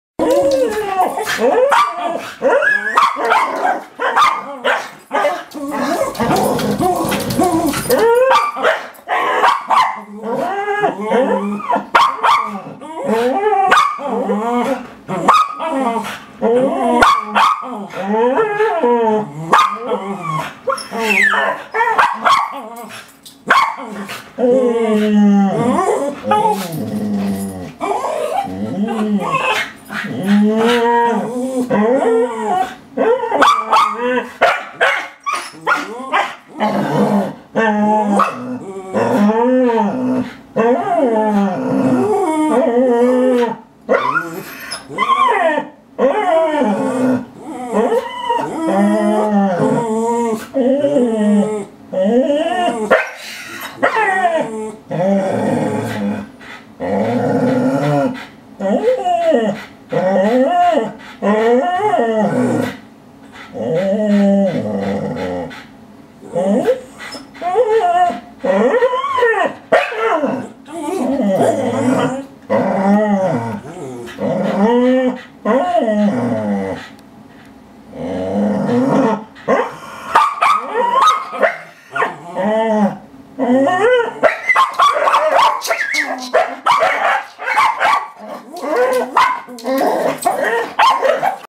Opossum drinking from the dog's water dish
The other day, we found this little guy pretending to be dead in the yard.  We relocated him outside the fence but he came back, as the video shows, to enjoy our water dish.  Our dogs were very interested in him, as you can tell from all of their whining that you can hear.